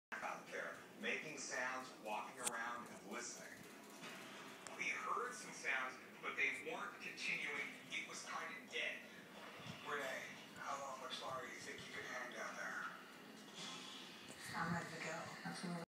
Chameleon two different colors
Just a short clip of my lil guy out before cage cleaning